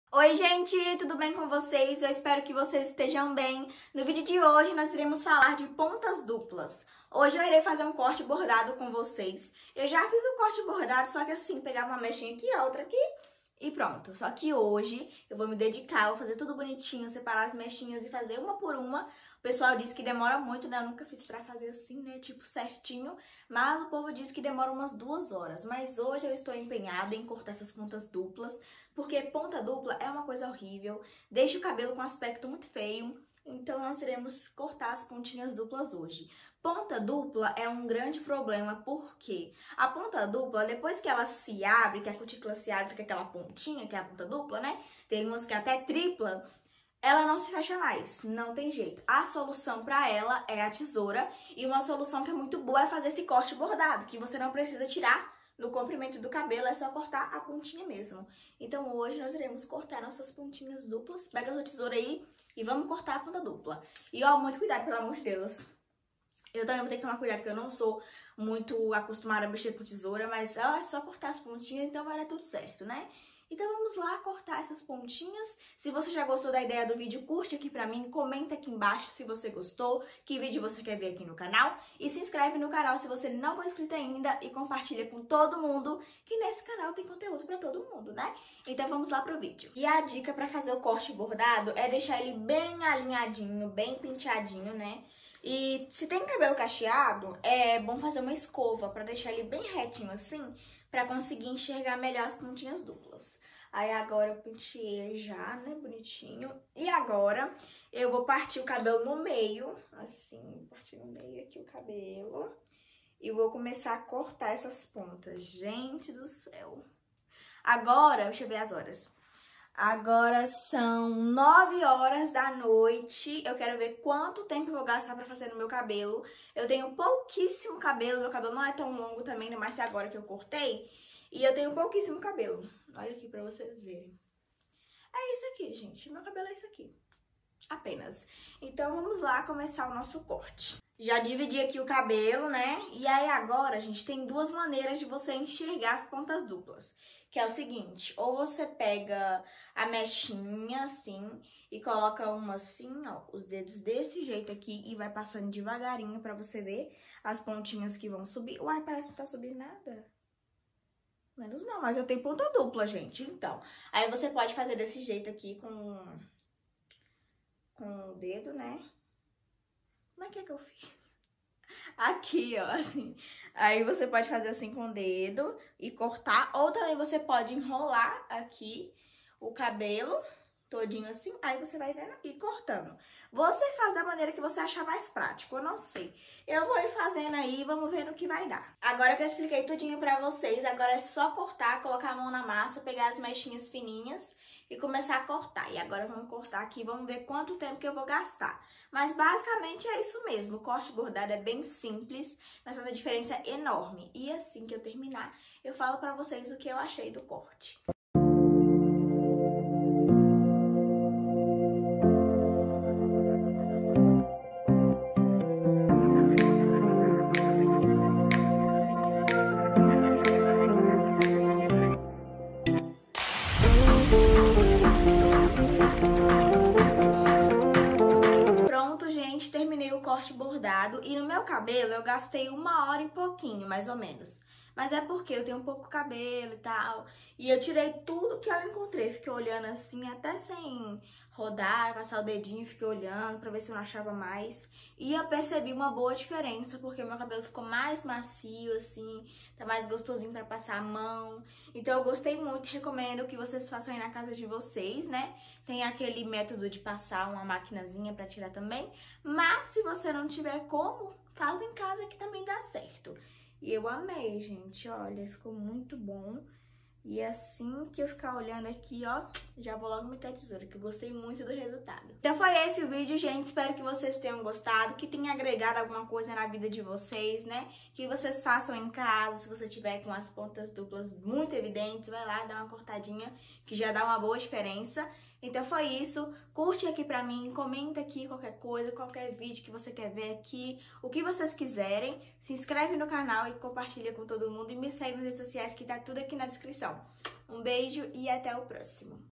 CORTE BORDADO EM CASA
Oii genteee! Tudo bem? No vídeo de hoje fiz um
Corte bordado no meu cabelo, espero que gostem, beijos e até o próximo.❤️